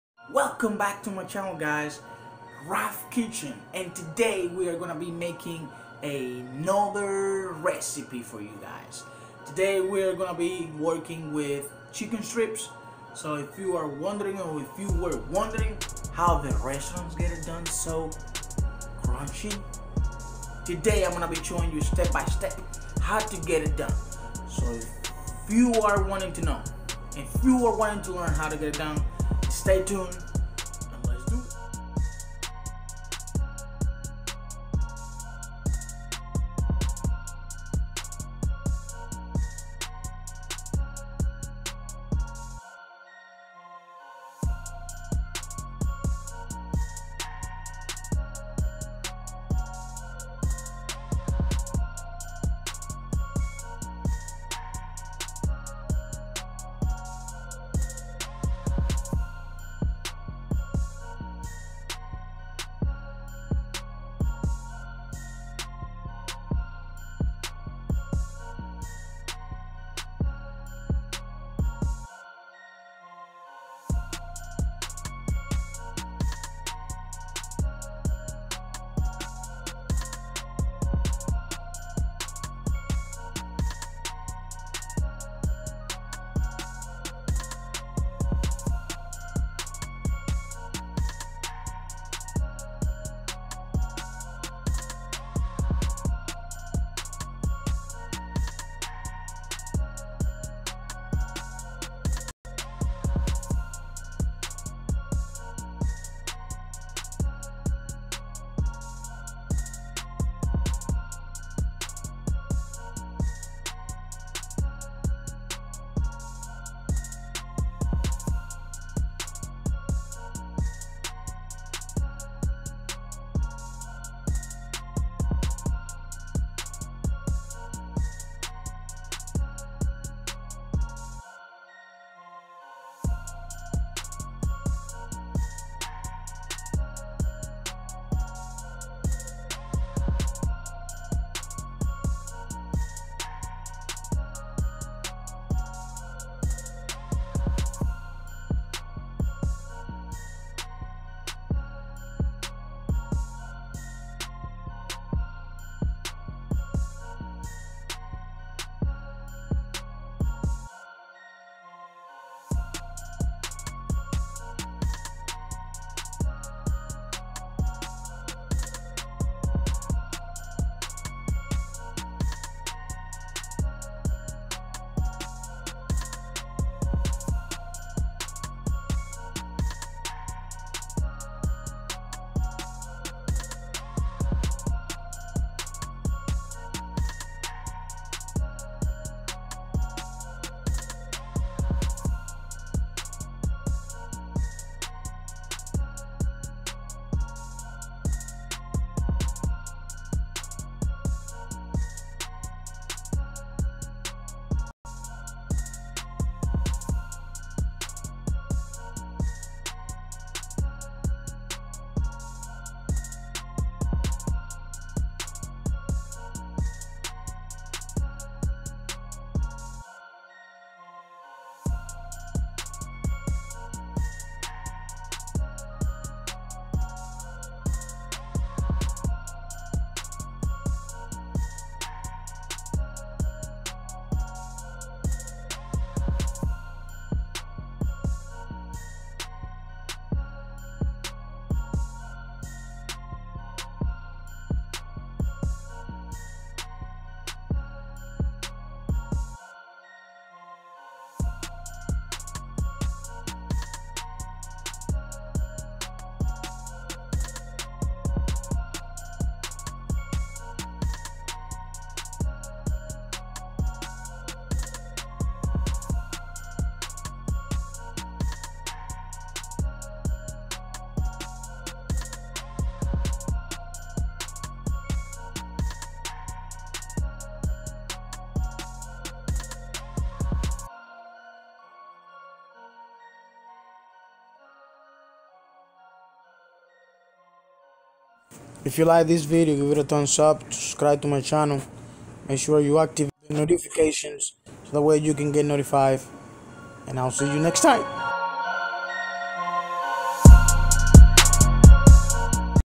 CHICKEN STRIPS. (Raf kitchen)
Ingredient.

Chiken
Oil
Flour
Corn starch
Peper
Oregano
Garlic pauder
Meat tenderize
Mustard
Salt
Black peper